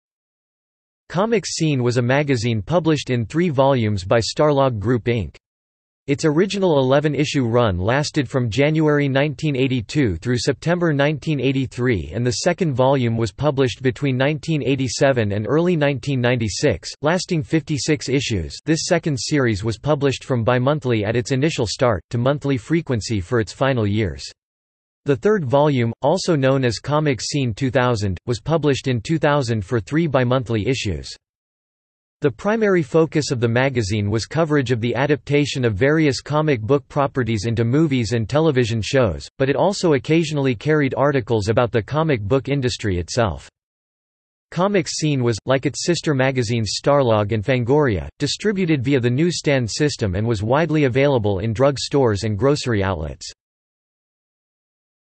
Comics Scene | Wikipedia audio article
- Socrates

SUMMARY
Comics Scene was a magazine published in three volumes by Starlog Group Inc. Its original 11-issue run lasted from January 1982 through September 1983 and the second volume was published between 1987 and early 1996, lasting 56 issues (this second series was published from bi-monthly at its initial start, to monthly frequency for its final years). The third volume, also known as Comics Scene 2000, was published in 2000 for three bi-monthly issues.
The primary focus of the magazine was coverage of the adaptation of various comic book properties into movies and television shows, but it also occasionally carried articles about the comic book industry itself.
Comics Scene was, like its sister magazines Starlog and Fangoria, distributed via the newsstand system and was widely available in drug stores and grocery outlets.